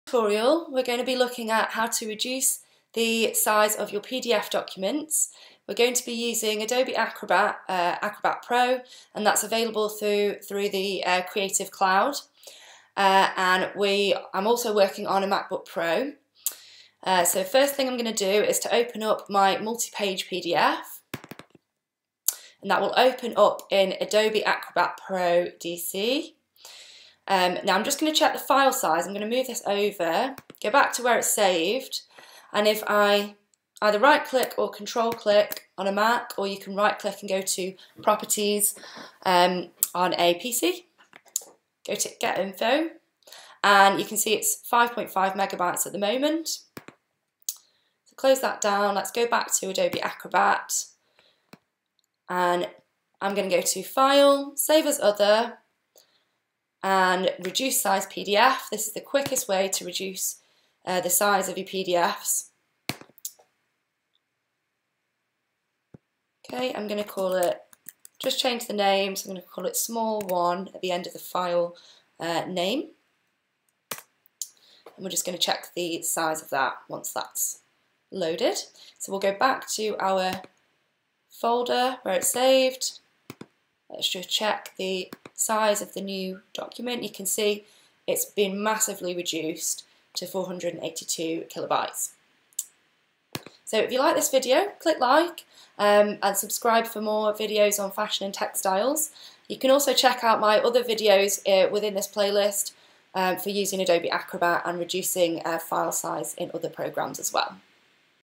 PDF File Size Reduction - Adobe Acrobat
Using 'File - Save As Other - Reduced Size PDF' in Acrobat to reduce file sizes.